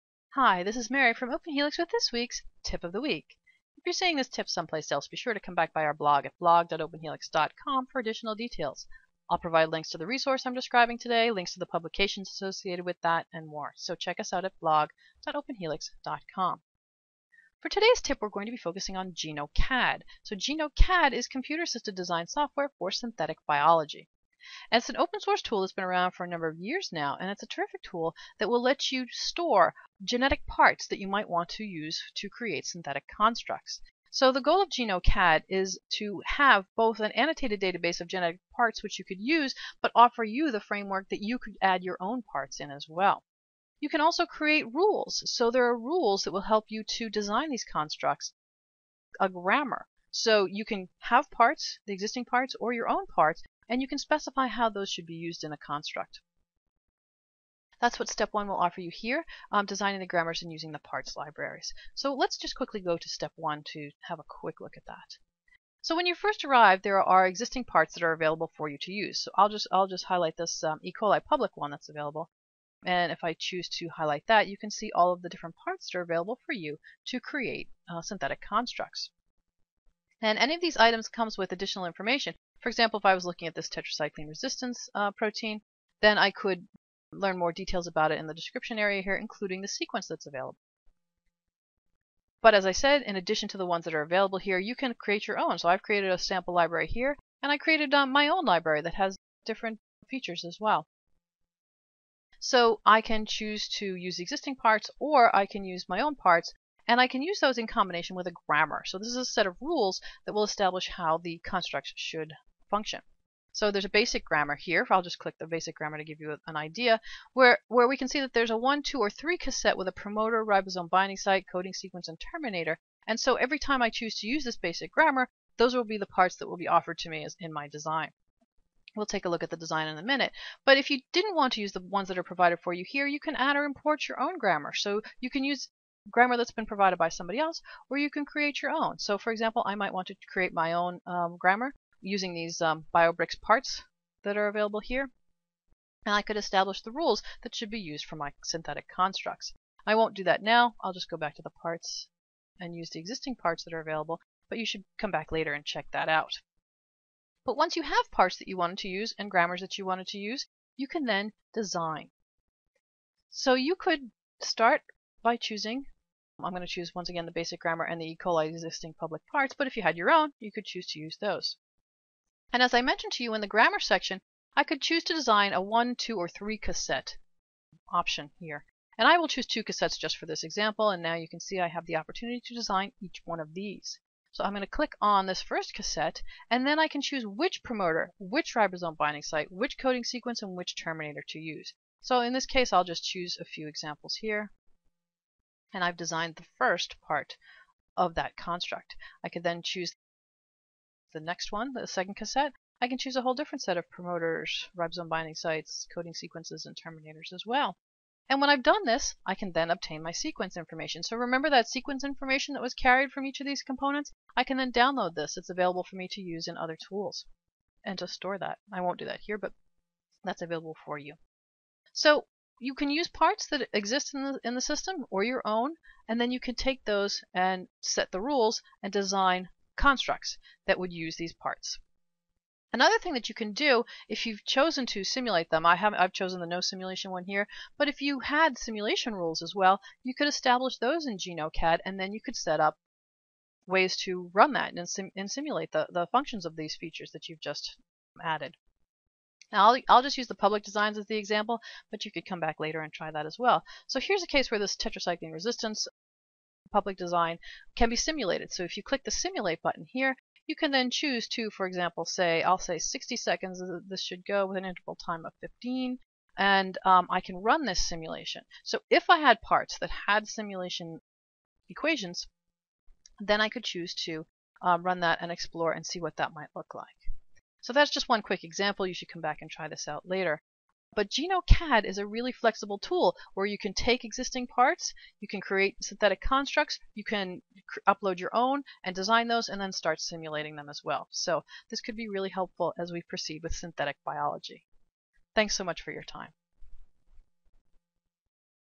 Video Tip of the Week: GenoCAD for synthetic biology
GenoCAD for computer-aided design for synthetic biology is an open-source software tool that will help you to store, assemble, and simulate designs for new sequences.